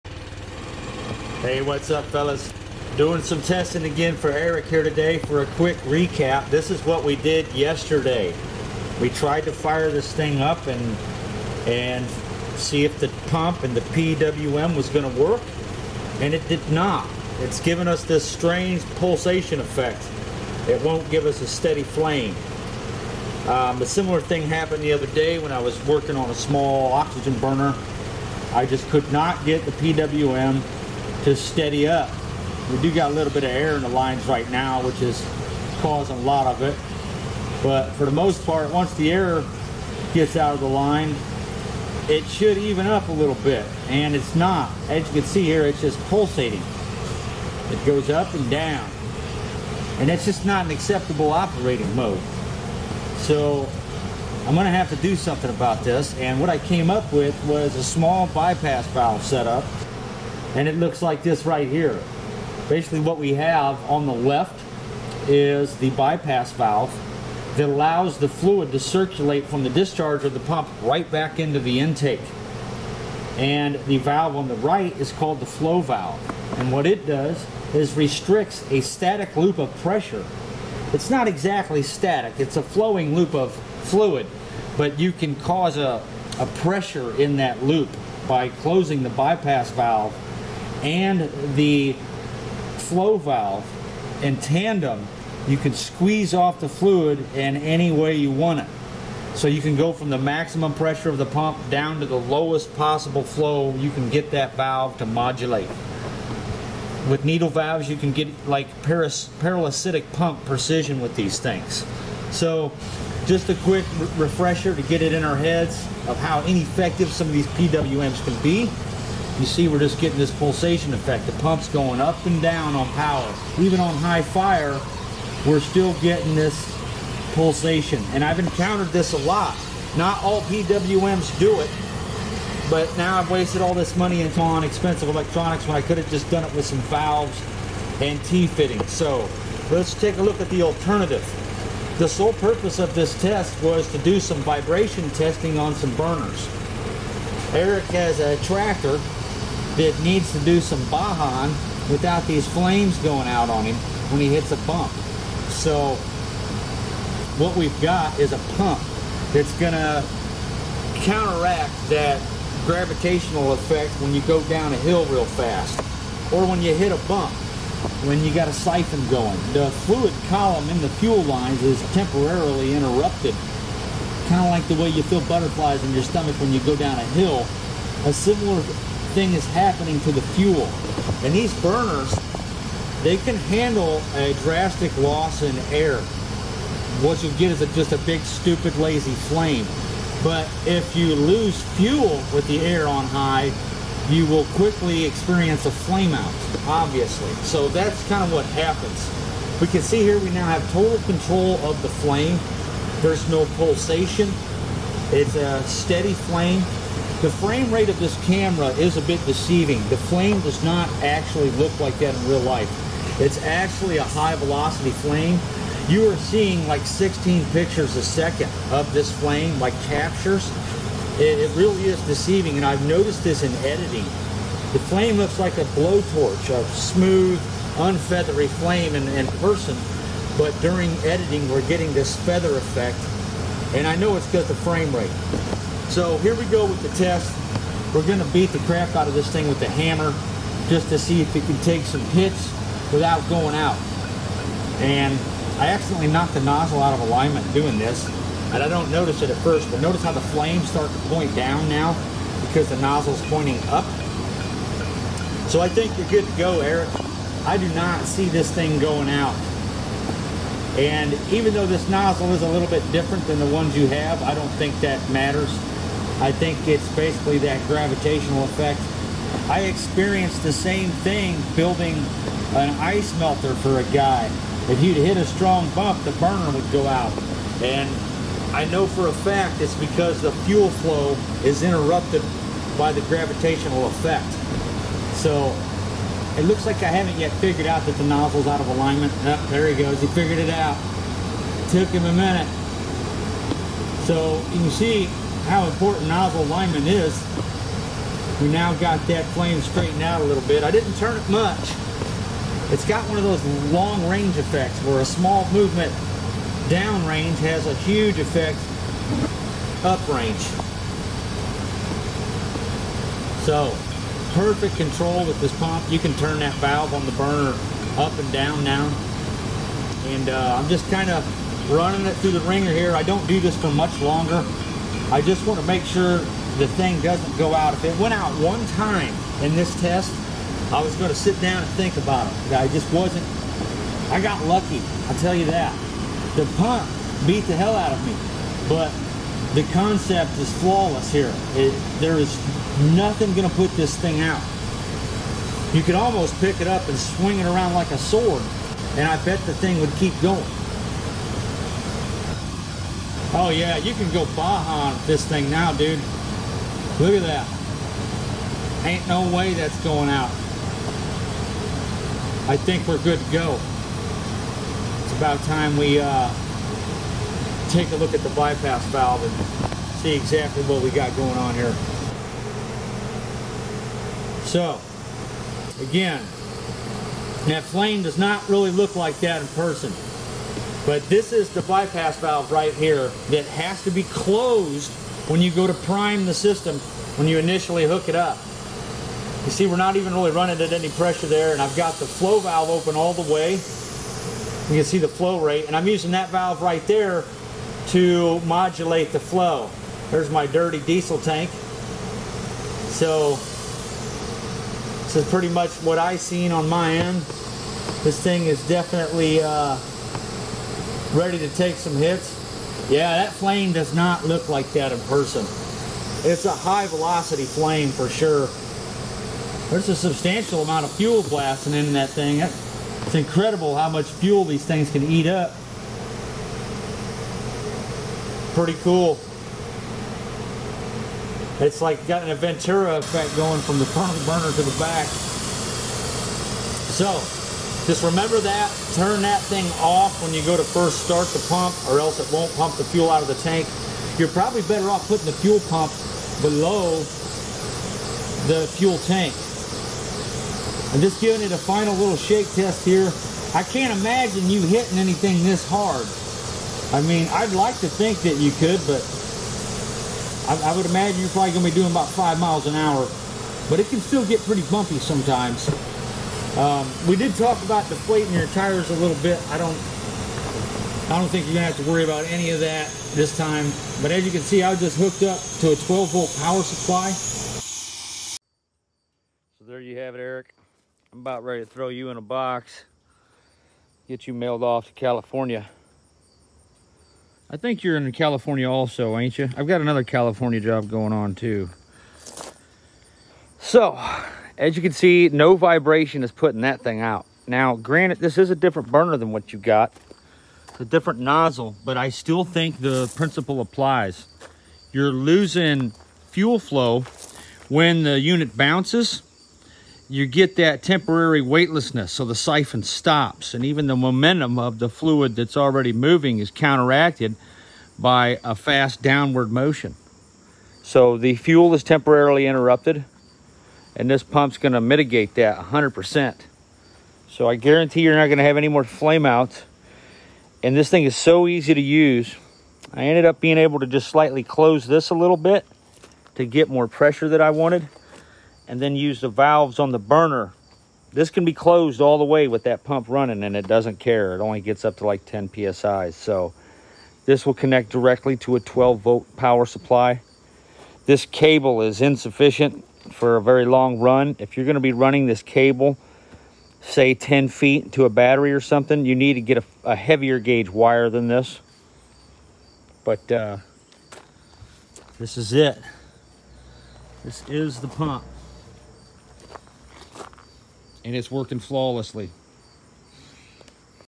High vibration agriculture Burner using bypass pump
Mastering fuel and oil pumps flow rate can be a game changer when it comes to saving money and adding durability to a design .